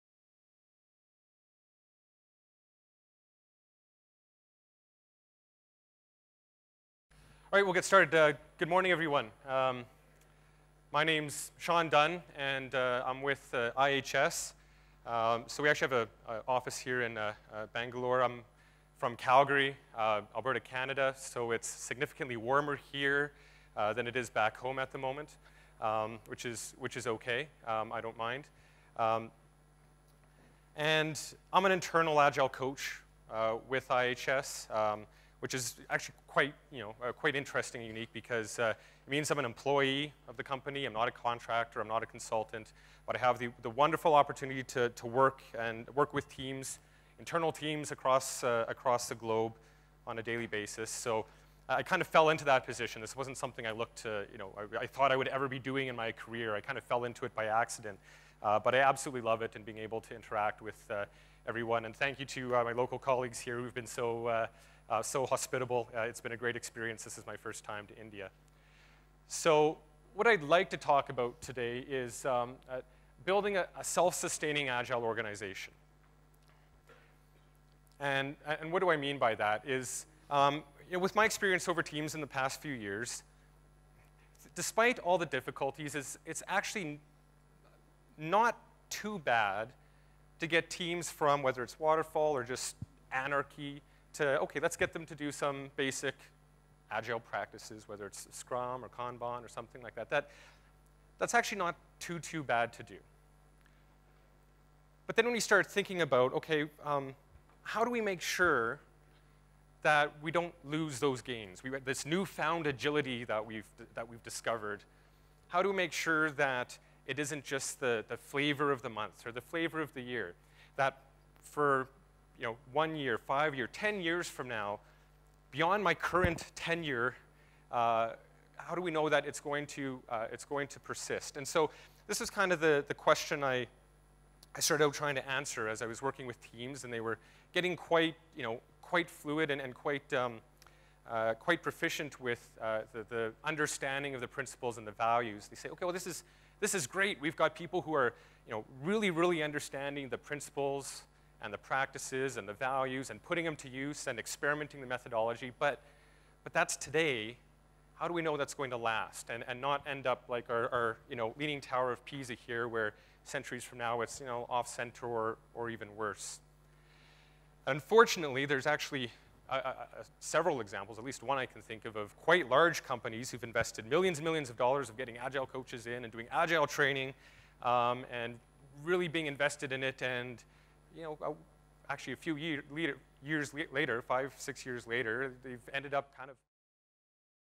Building a Self-Sustaining Agile Organization: An Exercise in Leadership by Sean Dunn
A successful agile transformation is a challenge - so how to ensure that these gains will be resilient and sustain over time? How can one be sure that the agile values and principles will be passed on to future generations? What characteristics differentiate the agile organization that is successful today and the one that will continue to be successful well into the future?

This lecture leverages Sean's 13 years of military experience to explore how leaders deliberately build great self-sustaining organizations.  Leaning on first-hand case studies from coaching dozens of agile teams, learn about the leadership behaviours that build self-sustaining cultures, and those which fail to see beyond just the methologies.